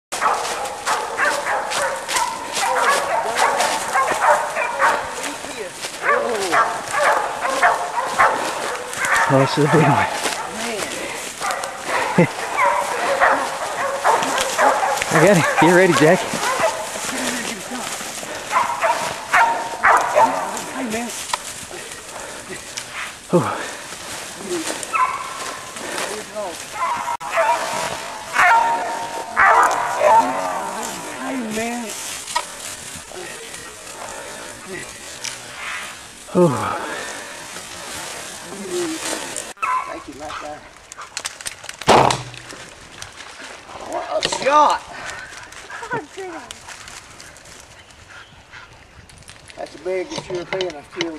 hog shot.wmv
Hog bow kill with rage and lumenok. Rage takes down a big hog in its tracks.